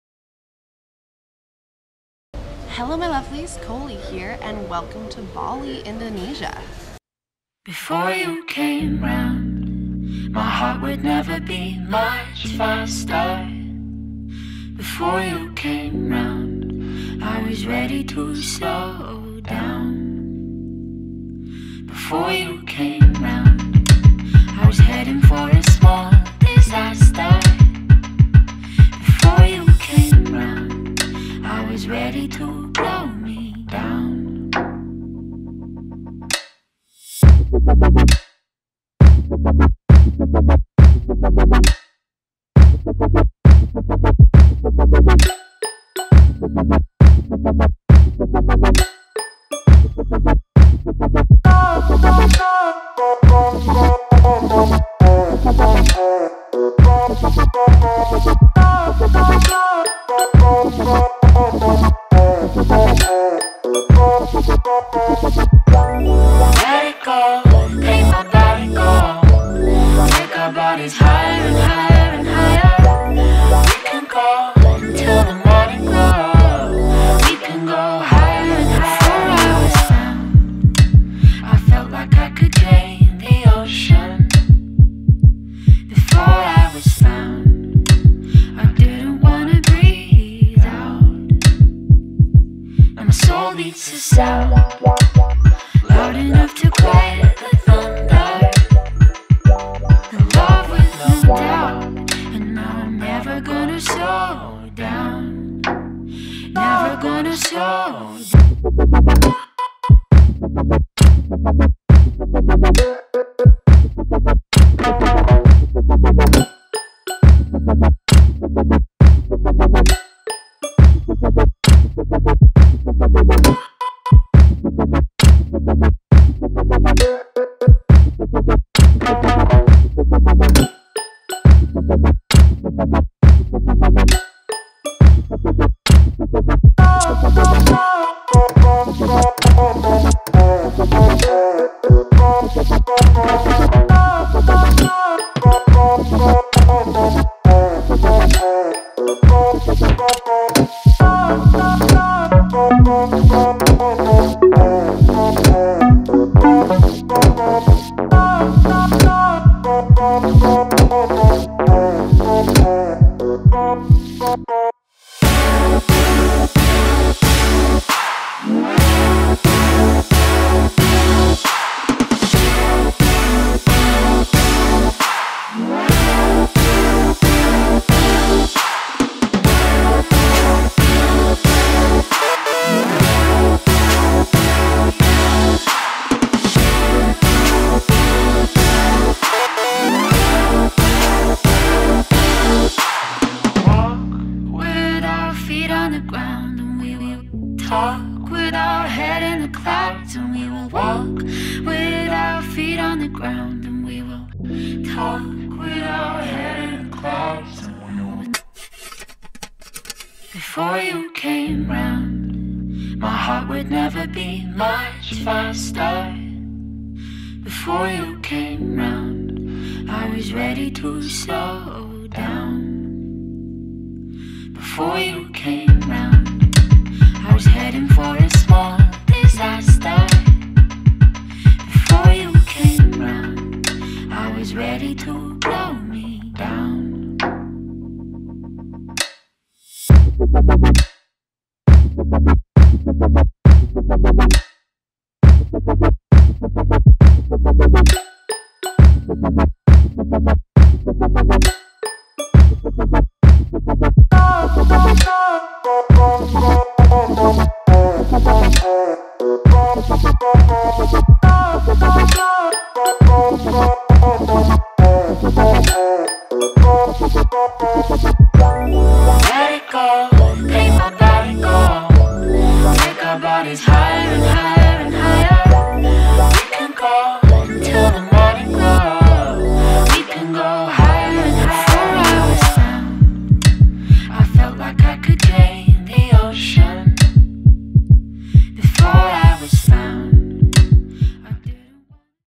From Land to Sea | BALI VLOG
OPEN FOR MORE INFO vvvv

BALI VLOG IS UP! I know this is super late but better late then never ;) Apologies for the shaky footage, I couldn't get my stabalizer to work in premiere. I hope you enjoy! xx

xoxo,
C